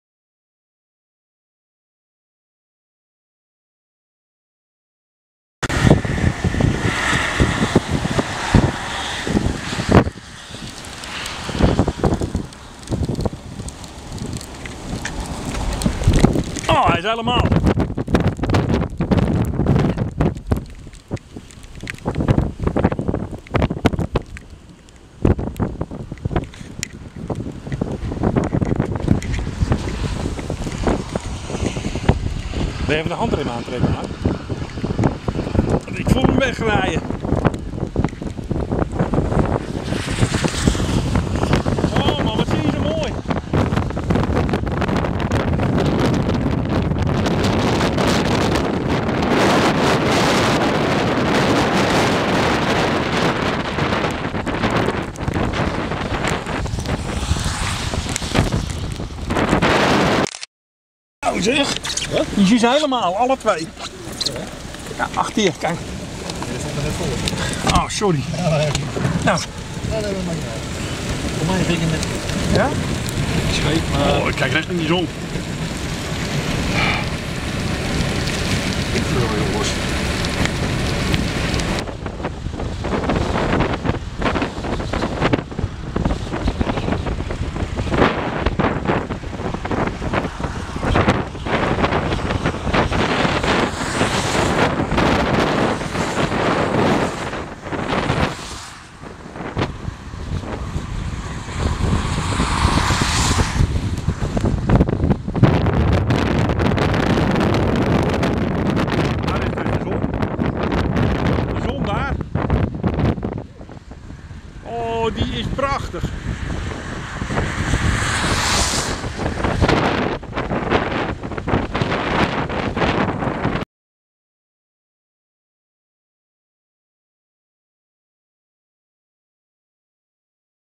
chase 24 augustus 2010 deel 2
Deel 2 van de chase. We stonden nu op de Oostvaardersdijk tussen Lelystad en Almere. Te genieten van het spectakel dat op ons af kwam.